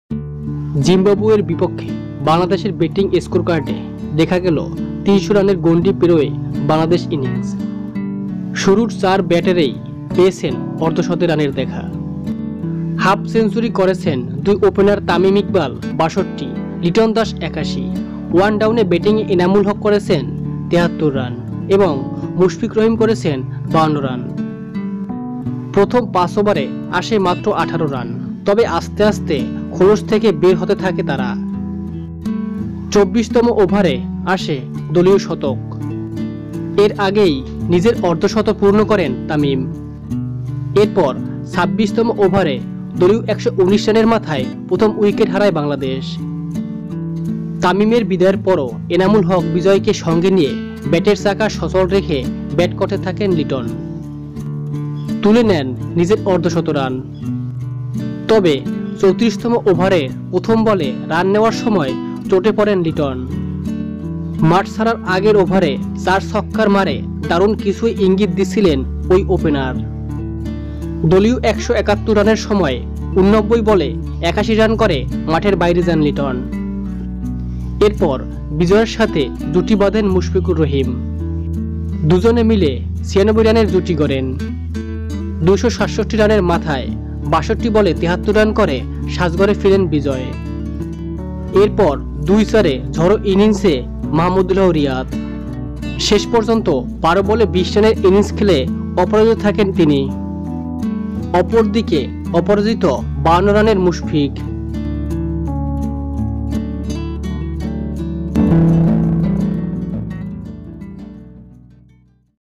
বাংলাদেশের চার ব্যাটারের অর্ধশত রানে ৩০০ পার|Cricket |Probaha news
জিম্বাবুয়ের বিপক্ষে বাংলাদেশের ব্যাটিং  স্কোরবোর্ডে ৩০০রানের গণ্ডি পার। শুরুর ৪ ব্যাটারই পেলেন অর্ধশত রান।

Bangladesh crossed the 300-run barrier on the batting scoreboard against Zimbabwe.  The first 4 batters got fifty runs.

Welcome to Probaha news   Youtube Channel.